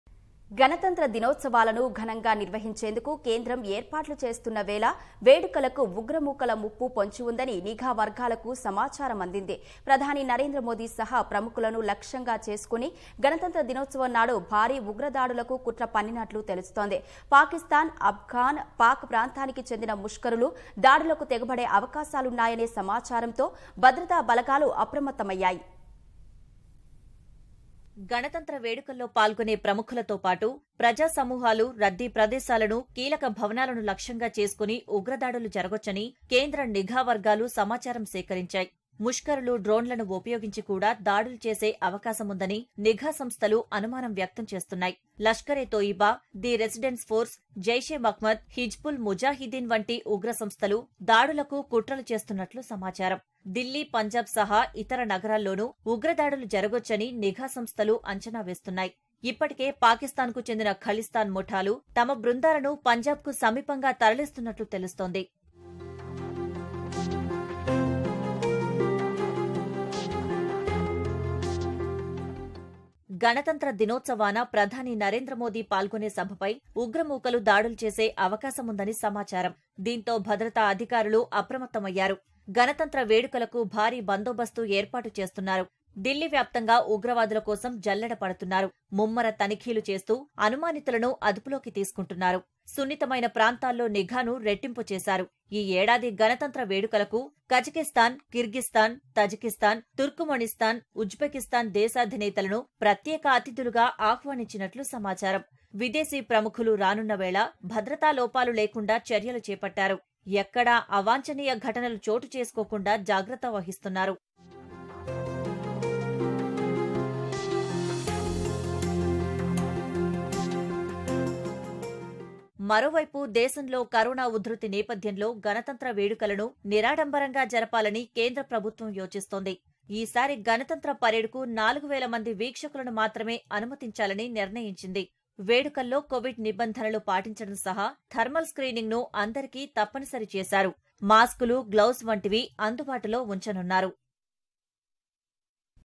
There is a Possibility of Terrorist Attacks Targeting VIP's on Republic Day: Intelligence Sources
గణతంత్ర దినోత్సవాలను ఘనంగా నిర్వహించేందుకు కేంద్రం ఏర్పాట్లు చేస్తున్న వేళ
వేడుకలకు ఉగ్రమూకల ముప్పు పొంచి ఉందని... నిఘా వర్గాలకు సమాచారం అందింది.
ప్రధాని నరేంద్రమోదీ సహా ప్రముఖులను లక్ష్యంగా చేసుకుని గణతంత్ర దినోత్సవం నాడు భారీ ఉగ్రదాడులకు కుట్ర పన్నినట్లు. తెలుస్తోంది. పాకిస్థాన్ , అఫ్గాన్ -పాక్ ప్రాంతానికి చెందిన ముష్కరులు దాడులకు తెగబడే అవకాశాలున్నాయనే సమాచారంతో. భద్రతా బలగాలు అప్రమత్తమయ్యాయి.